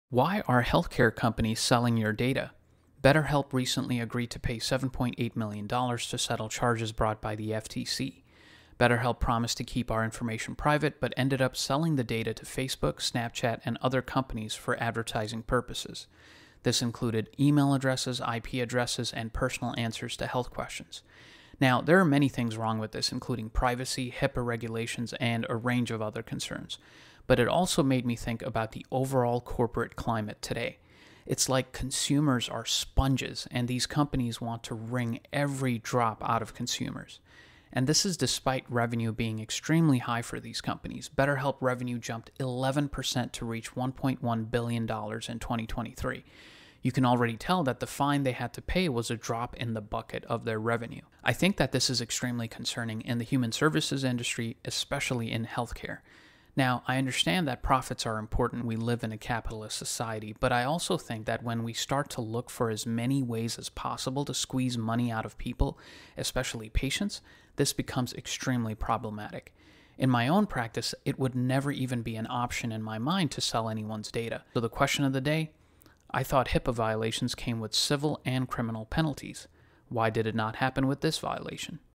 Why are Healthcare companies selling your data?
Dr. Jerome Frank talked about the healing relationship between the physician and the patient. Don't these types of predatory practices disrupt that relationship?